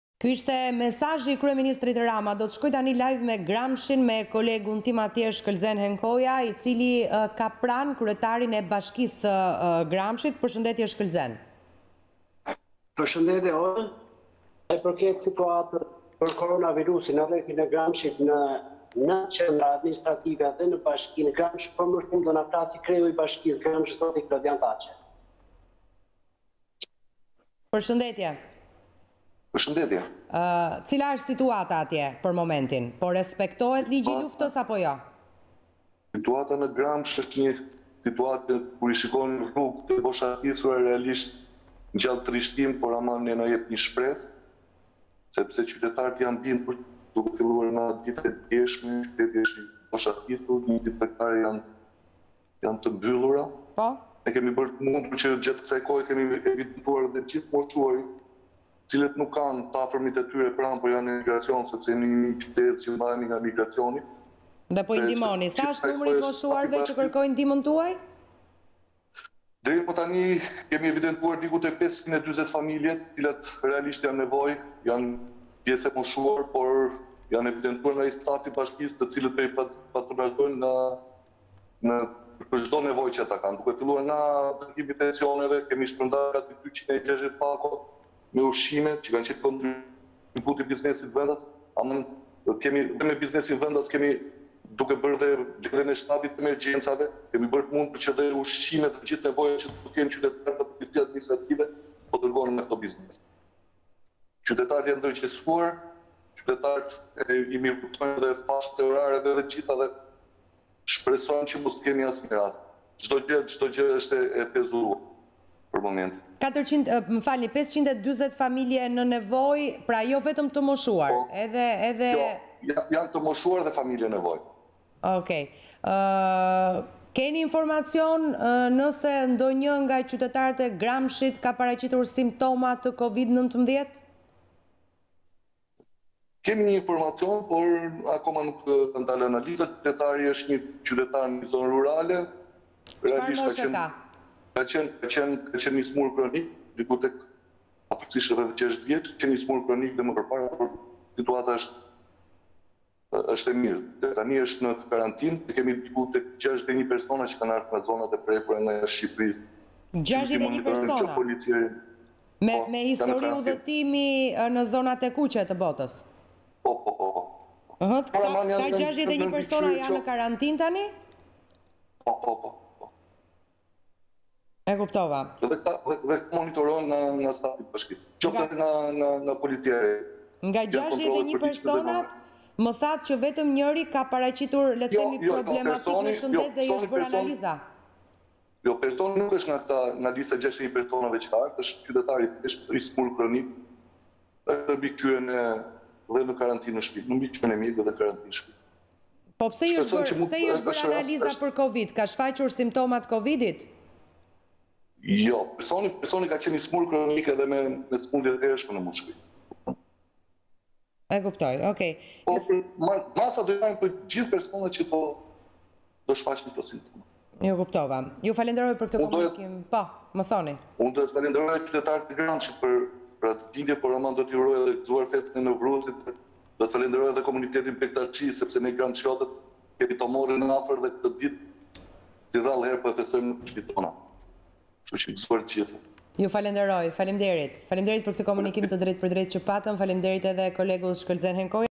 Koronavirus/Bashkiaku i Gramshit, Taçe: 540 familje në nevojë do ndihmohen.Qytetaret i binden masave
Koronavirusi/ Bashkiaku i Gramshit, Taçe: 540 familje në nevojë do ndihmohen. Qytetaret i binden masave